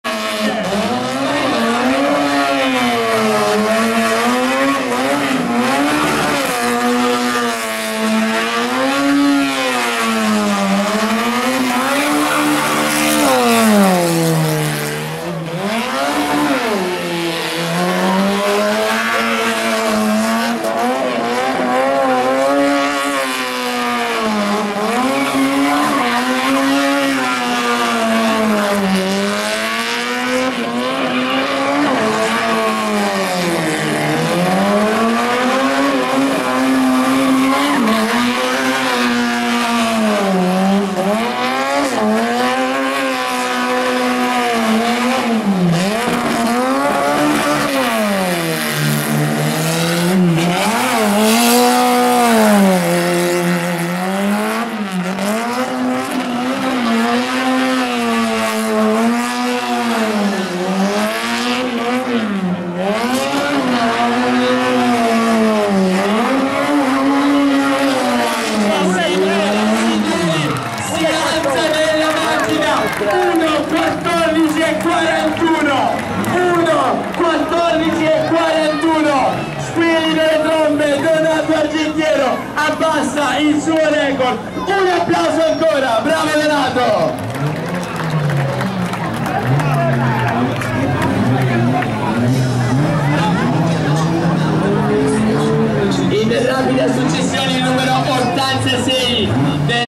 CGP2014 - 28^ Gimkana Cegliese - Miglior tempo assoluto Donato Argentiero 112
Il miglior crono ottenuto da chi se non Donato Argentiero, il numero 1 del team Silver Car Motorsport infatti è andato forte riuscendo ad abbassare tutti i migliori tempi di giornata, suoi, 1.16 su 500, 15 x due giri su 127, 1.18 su 128, con questo giro 1.14, impensabile per gli altri concorrenti. Grande show che fa esaltare il pubblico